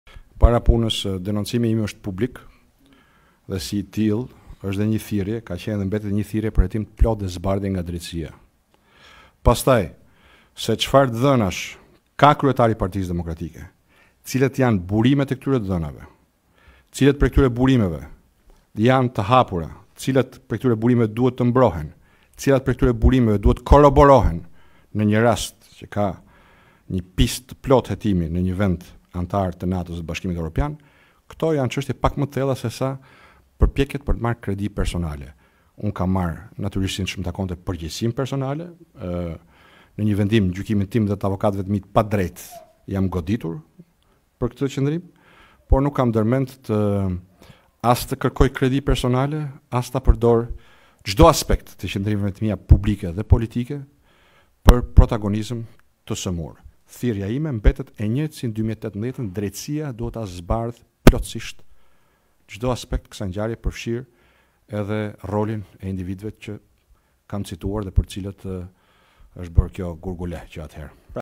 Olsi Rama i përfshirë në trafikun e kokainës në Xibrakë? Basha: E kam denoncuar i pari ngjarjen
Kryetari i Partisë Demokratike Lulzim Basha, në një konferencë për mediat, u pyet në lidhje me akuzat e Gazment Bardhit në drejtim të Olsi Ramës, si i dyshuar se është përfshirë në trafikun e kokainës në Xibrakë.

Basha theksoi se ka denoncuar i pari ngjarjen që më 2018, duke shtuar se bën thirrje që gjithçka të hetohet.

Ai shtoi se ndryshe nga se veprojnë të tjerët, vetë si person nuk krekoset.

“Unë e kam denoncuar këtë skandal i pari, që duhet hetuar, atë për përfshirjen e mundshme të Olsi Ramës në trafik droge në Xibrakë.

Nuk e përdor çdo aspekt të qëndrimeve politike publike për interesa vetjake.

Thirrja ime mbetet e njëjtë si më 2018, që të hetohet ngjarja.

Se çfarë kanë bërë me këto prova e me këto indicie, i takon drejtësisë.

Nuk kam pse krekosem para mediave, unë kam denoncuar publikisht.

Nuk kam rrahur gjoksin para kamerave, denoncimi është bërë.

Nuk bëra denoncim për të hequr vëmendjen nga ndonjë çështje.

Të tilla kanë qenë përpjekjet tona edhe për dosje të tjera”, tha Basha.

I pyetur se përse e ka mbështetur marrëveshjen e 2008, Basha deklaroi se ka menduar në atë kohë se kjo marrëveshje do të linte kriminelët jashtë listës, por se ka gabuar në këtë perceptim.

“Qëndrimet e mia për marrëveshjen e vitit 2008 kanë nisur prej kohësh.

Unë kërkoj ndjesë para shqiptarëve, pasi kam qenë pjesë e atij grupi që kam votuar pro marrëveshjes, sepse donim të ndalonim personat me rekorde kriminale që të depërtojnë në politikë, por kjo nuk doli të jetë e vërtetë.

Vettingu i politikanëve është i domosdoshëm.

Kurse marrëveshja e 2017 u pranua nga të gjitha palët”, u shpreh Basha.